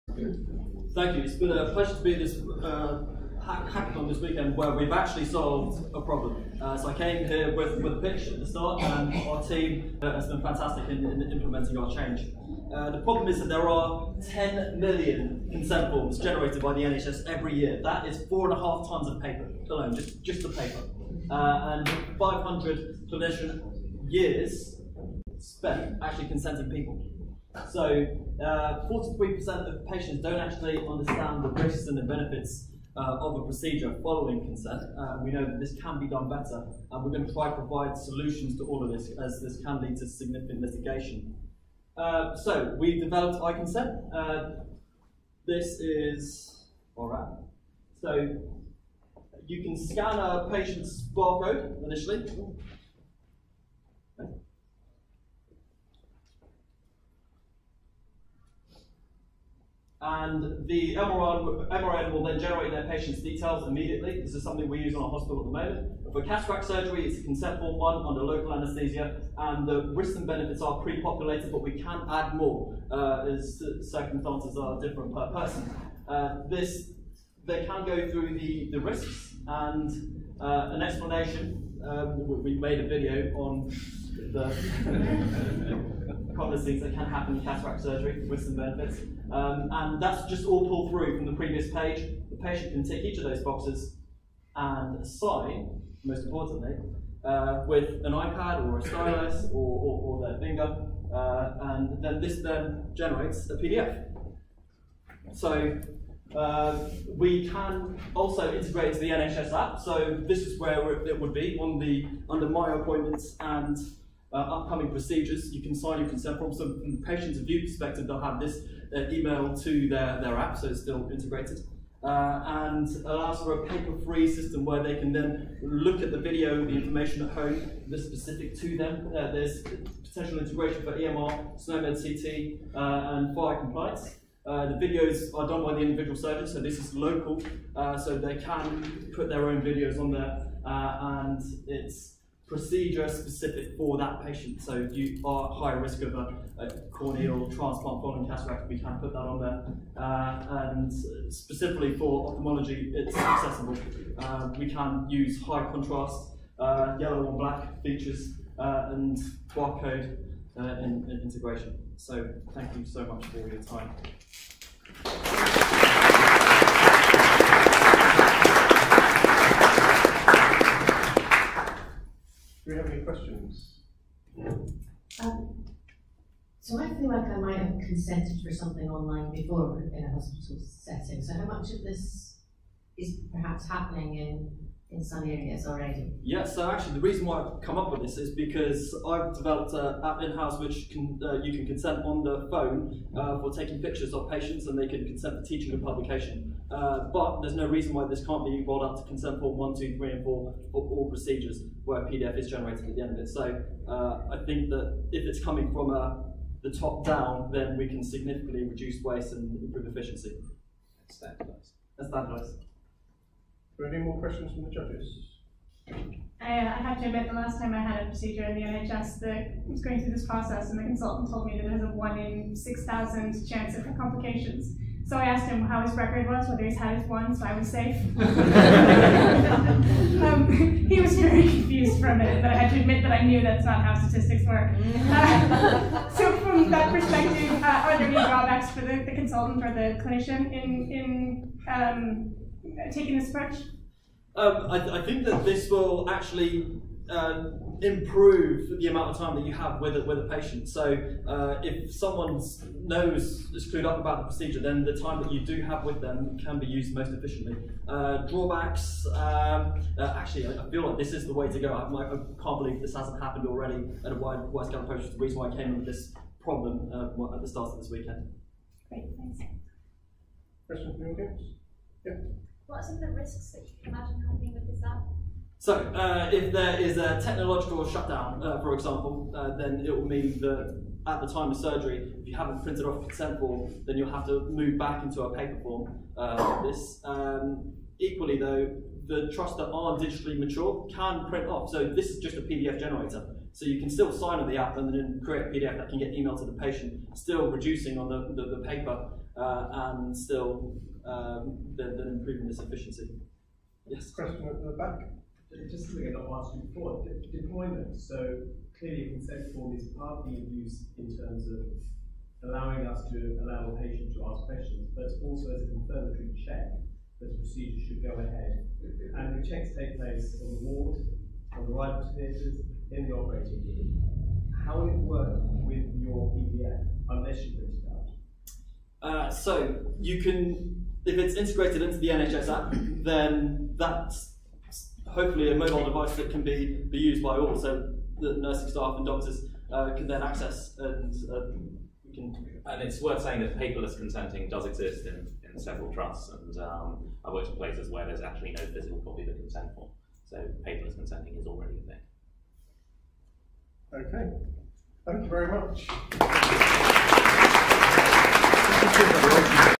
NHS Hack Day #22 Presentations: iConsent
Recording of the "iConsent" presentation from NHS Hackday #22 in London June 2019.